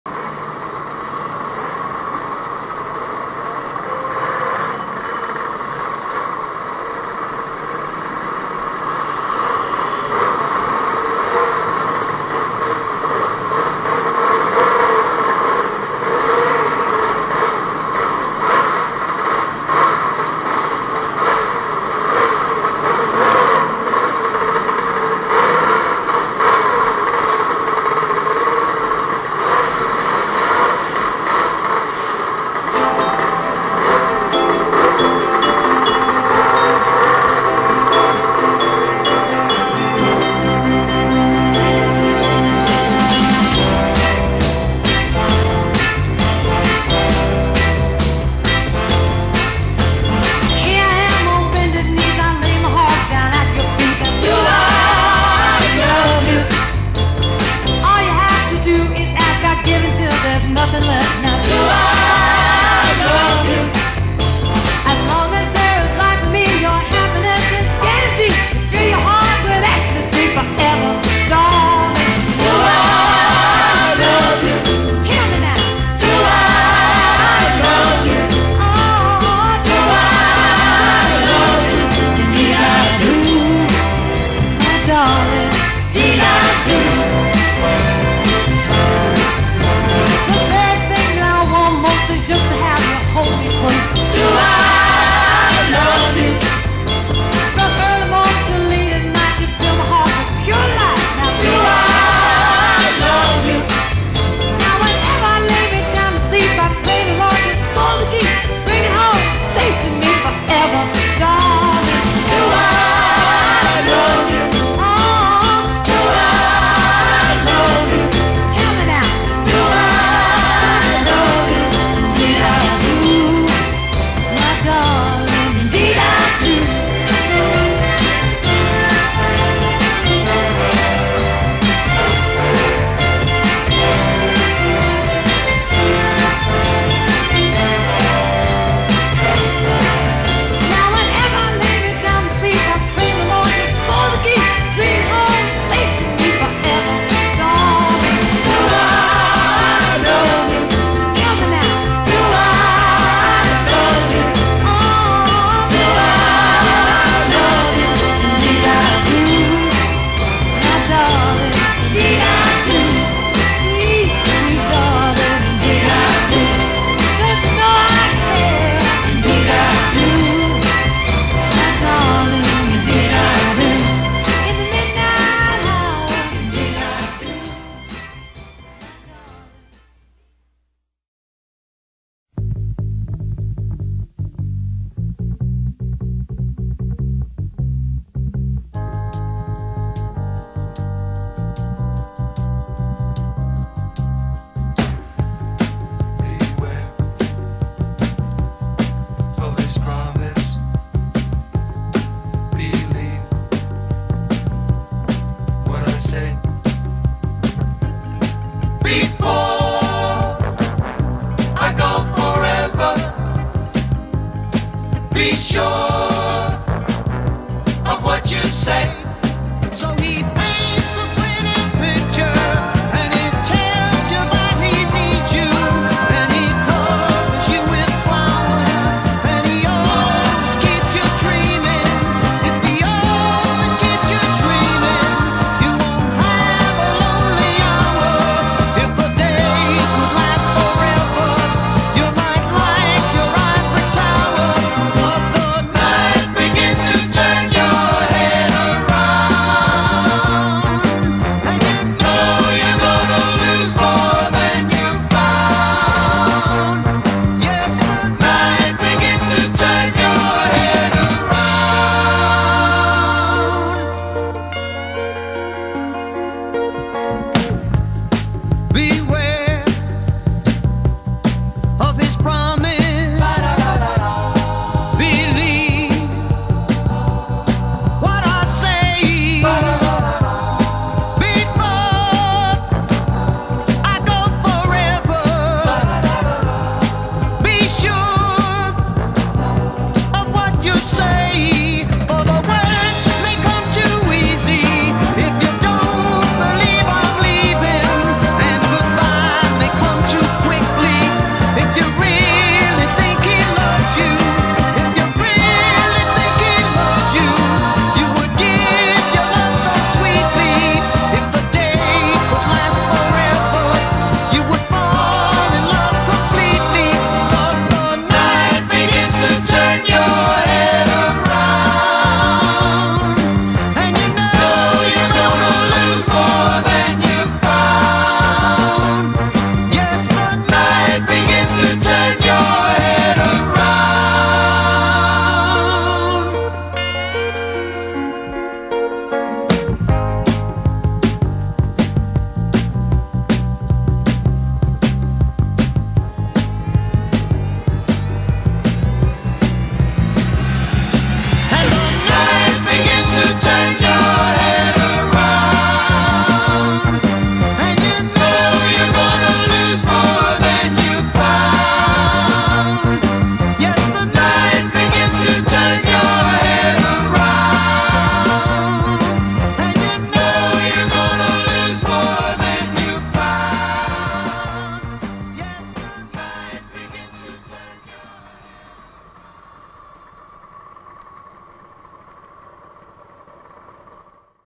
Isle of Wight Scooter Rally 2019
Some footage from Augusts bank holiday 2019 Isle of Wight Scooter Rally filmed on my budget helmet cam (sorry about the excess movement)